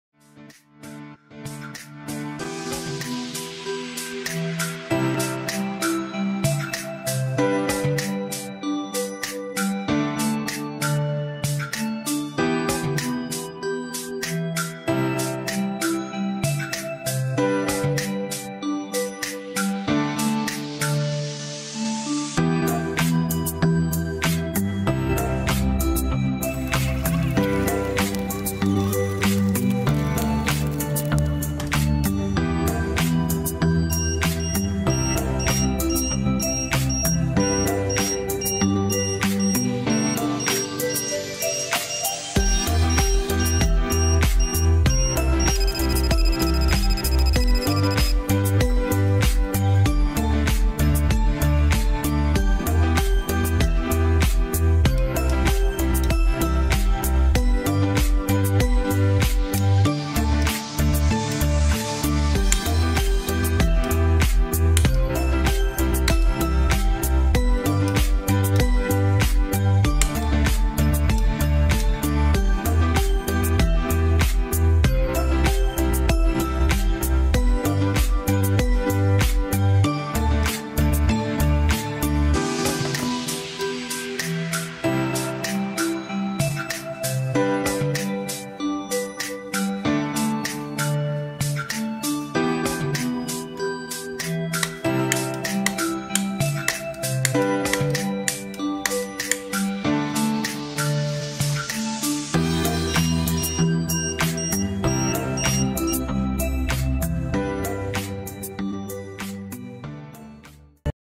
Wifi Light Switch. work with Tuya app.Voice control via amazon or Google Home.
You can see product details and how to set up in this vedio.Please let us know if you have more questions.If you want to buy or get more information, .